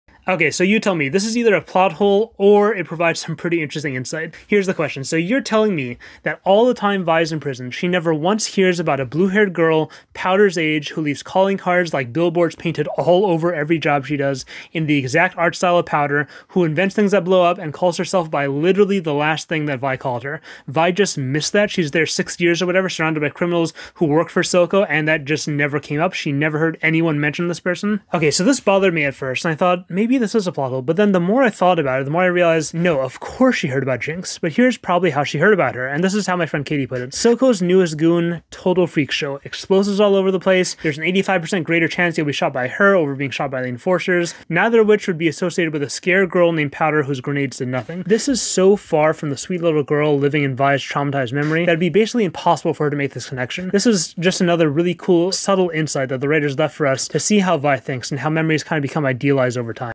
Why Didn't Vi Know About Jinx? (Arcane theory)
Tell me if you think this is a plot hole in Arcane.  Vi is in Stillwater for 6-7 years surrounded by underworld criminals (who mostly work for Silco), and somehow she doesn’t hear about a blue-haired girl the exact age of her sister who draws animals and monsters with colorful markers all over every job she does who calls herself JINX aka the last name Vi called her before they were separated?  How in the world did Vi not hear ANYTHING about the most theatrical girl criminal in Zaun?  Unless she DID hear about her, but there’s another reason she was surprised by Caitlyn’s photos…

Read my comic Minor Champion! NEW Chapter here!